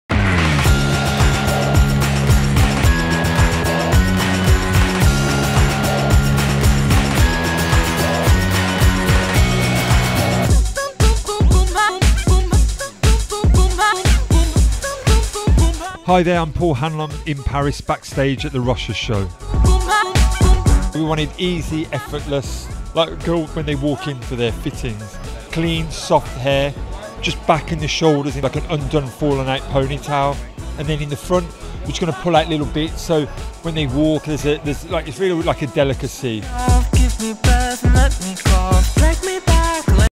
First Look at the Rochas Spring 2016 Runway Show Backstage in Paris | FashionTV
PARIS - This is the First Look from the Rochas Spring/Summer 2016 Runway Show at Paris Fashion Week! Exclusive interviews with the lead hair stylists and artists about the makeup and hair looks for the show, as well as interviews from FashionTV with the models and designer themselves. WATCH the video and let us know what you think. SEE it first on FashionTV !

With appearances by: Paul Hanlon, Molly Bair, Lucia Pieroni, Aya Jones, Maddison Stubbington, Sascha Lilic, Ellen Von Unwerth, Sophie Rochas, Marjan Jonkman, Daniel Falcao.